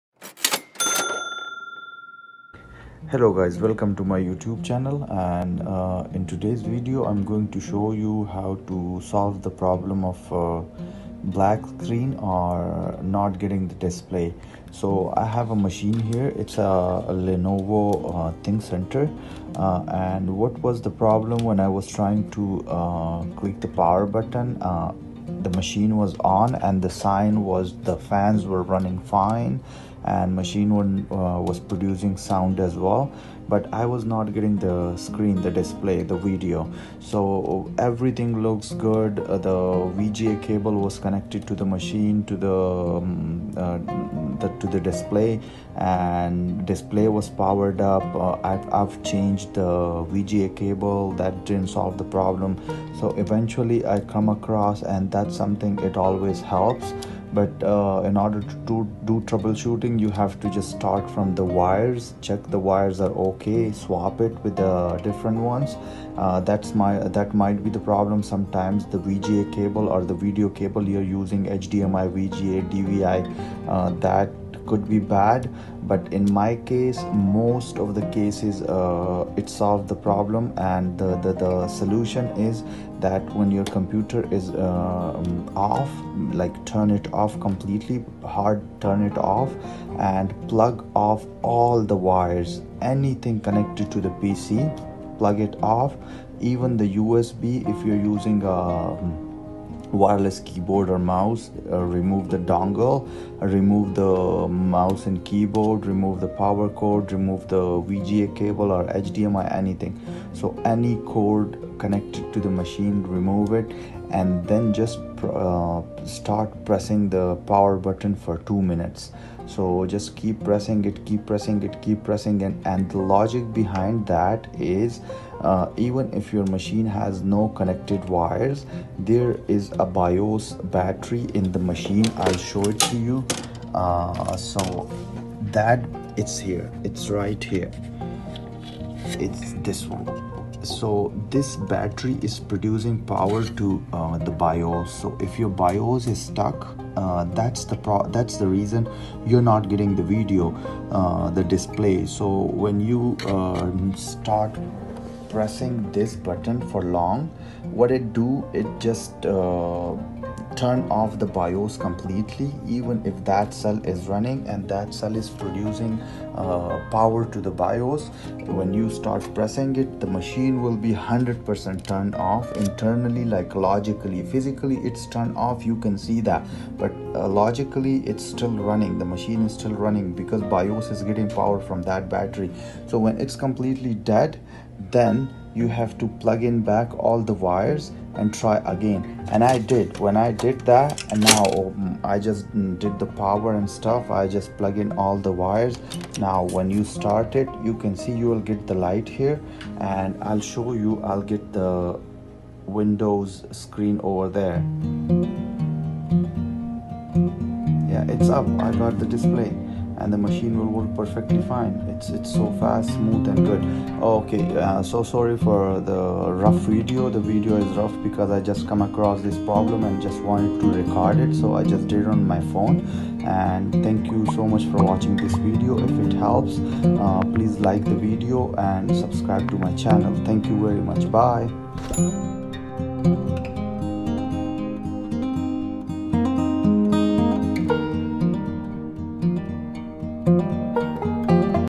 How to Fix Your PC Turns On but No Display Problem in Windows 10 / No Screen Signals / No Video
How to Fix Your PC Turns On but No Display  Problem in Windows 7, 8, 10 / No Screen Signals / No Video / Black Screen

Method 1. Test your monitor.
Method 2. Make sure your computer has completely restarted.
Method 3. Verify that the power supply voltage switch is set correctly.
Method 4. Perform a hard reset.
Method 5. Clear the BIOS memory.
Method 6. Reseat the memory modules.
Method 7. Understand LED lights.
Method 8. Check Hardware.